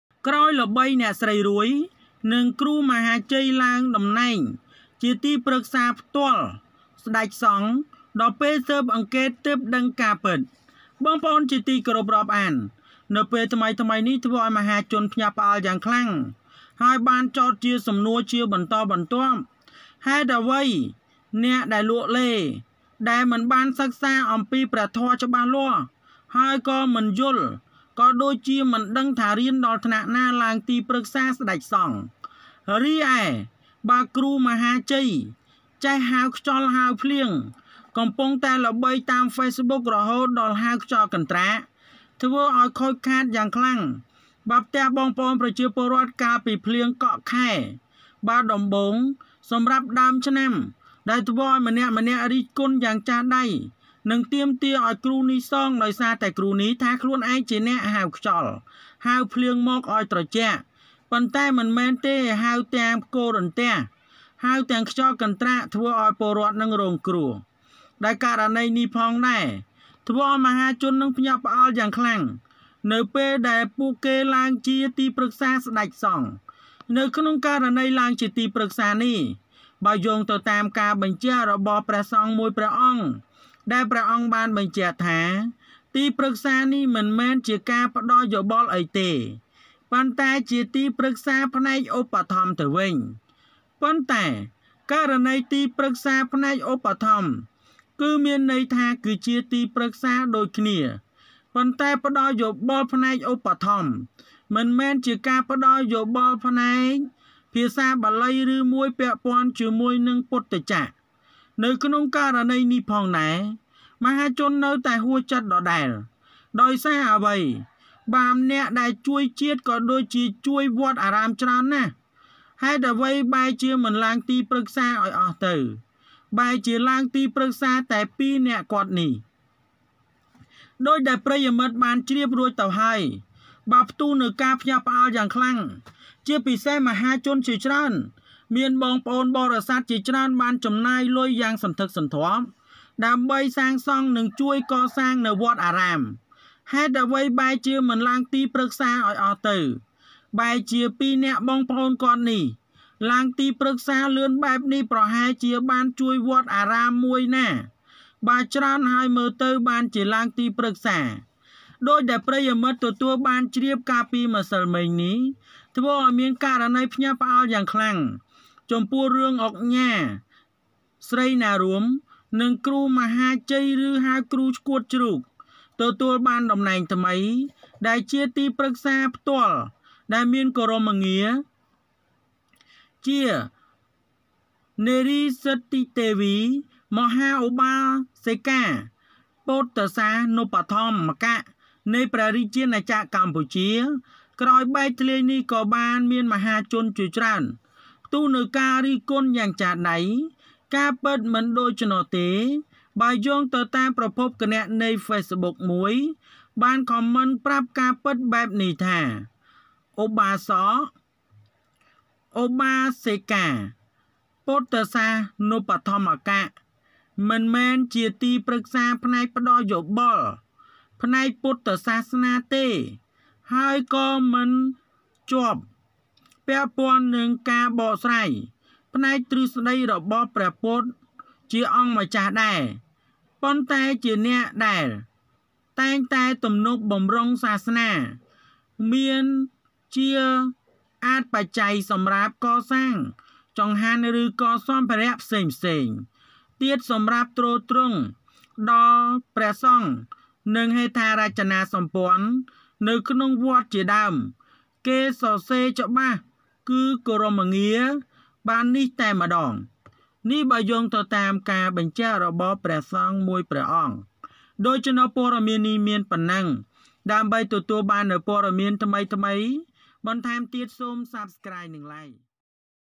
ច្បាស់ការហើយ! ក្រោយល្បី អ្នកស្រី រួយ និងគ្រូមហាជ័យ ឡើងដំណឹងជាទីប្រឹក្សាផ្ទាល់ សម្តេចសង្ឃដល់ស៊ើបៗដឹងក
ច្បាស់ការហើយ! ក្រោយល្បី អ្នកស្រី រួយ និងគ្រូមហាជ័យ ឡើងដំណឹងជាទីប្រឹក្សាផ្ទាល់ សម្តេចសង្ឃដល់ស៊ើបៗដឹងការពិត Definitely!  After becoming famous, Ms. Ruoy and Krou Moha Chey became the personal advisers of Samdech Sang to the detectives to know the truth.